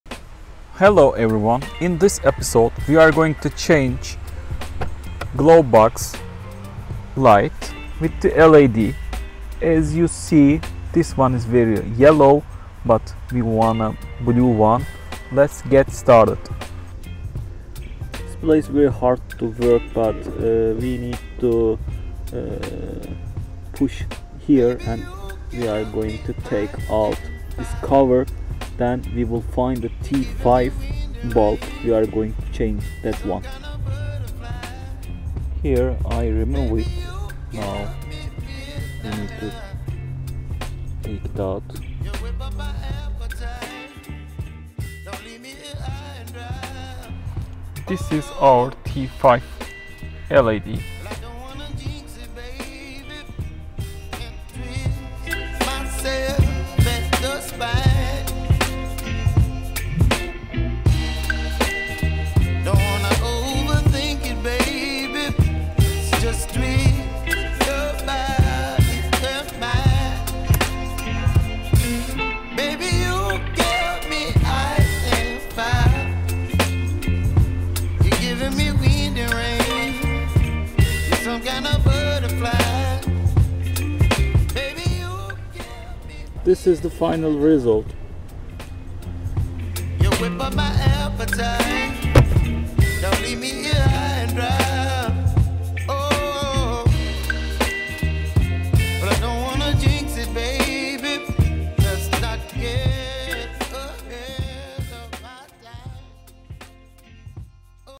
How to Replace Nissan Qashqai 2023 J12 E-Power Glove Box Lights w/ LED
Don't forget to "like" the video if you like it!

Replacement Glove Comparment Lights with LED bulbs
T5 LED Light Installation

Hello I am Necdet Akyürek! I produce many vlogs / videos about cars on my youtube channel.